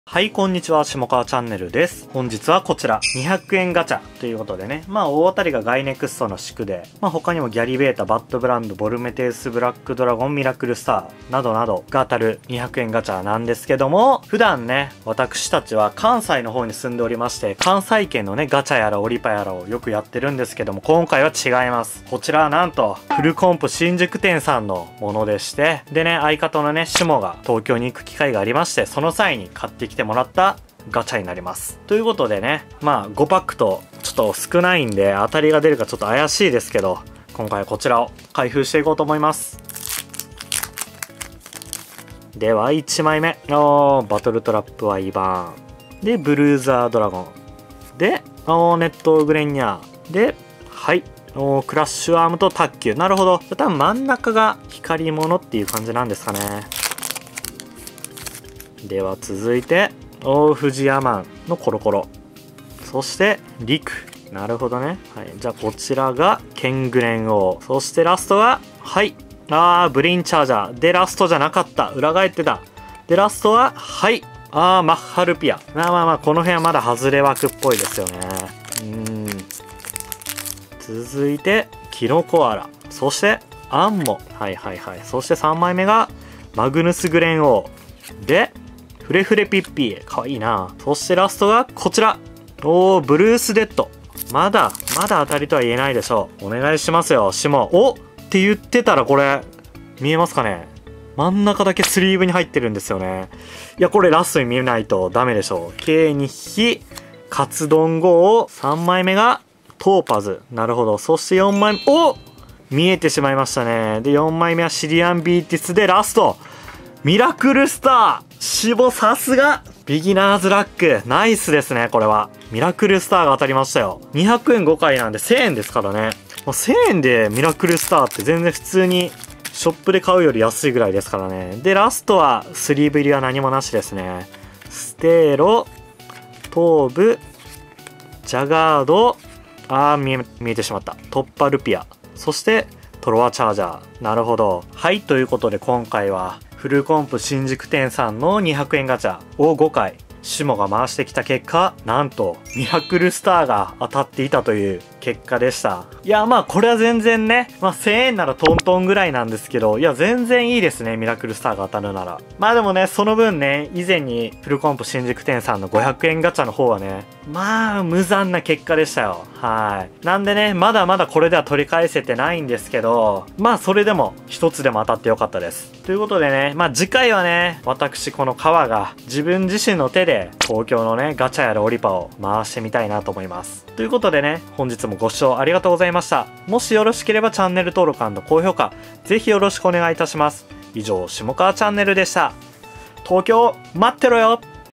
【デュエマ】東京のガチャってどうよ…！？相方の仇を！！【開封動画】
デュエルマスターズ200円ガチャ5口分の開封動画です！
(東京・フルコンプ新宿南口店)

・【デュエマ】女子が新宿500円ガチャ開封して、何が悪い！【開封動画】

デュエマ担当はカワです。

アカウント名：シモカワチャンネル